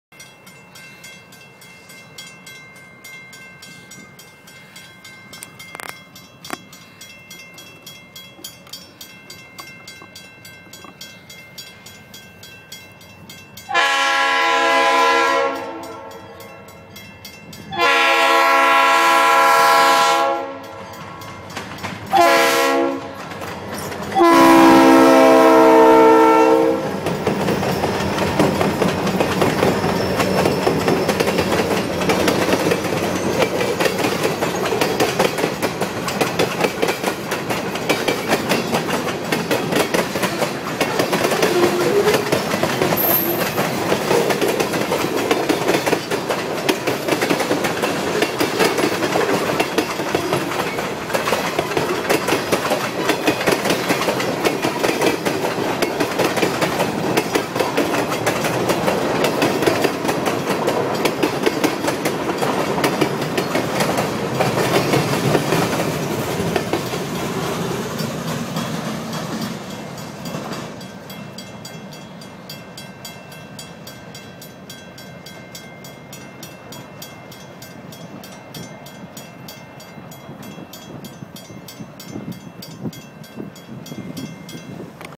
Short POED
Short Train passes through Tewksbury, MA.

Taped:4/24/2013